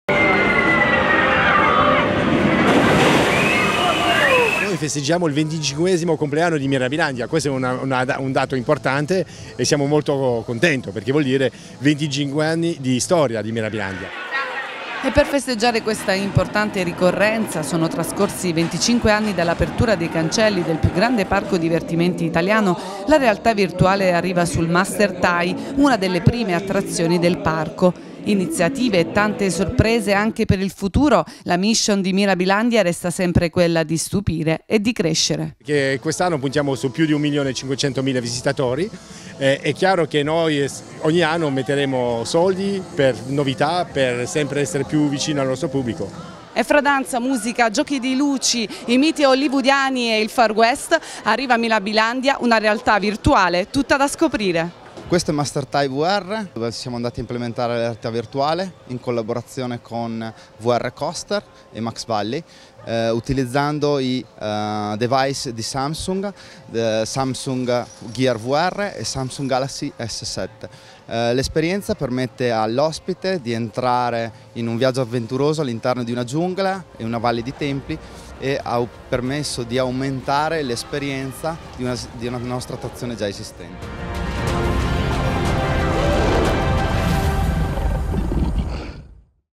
Ravenna: A Mirabilandia arrivano le montagne russe con la realtà virtuale | VIDEO
Crescono le presenze, gli spettacoli e le attrazioni di Mirabilandia. Il parco divertimento romagnolo, che quest’anno festeggia 25anni, è sempre più tecnologico. La realtà virtuale arriva anche sulle montagne russe.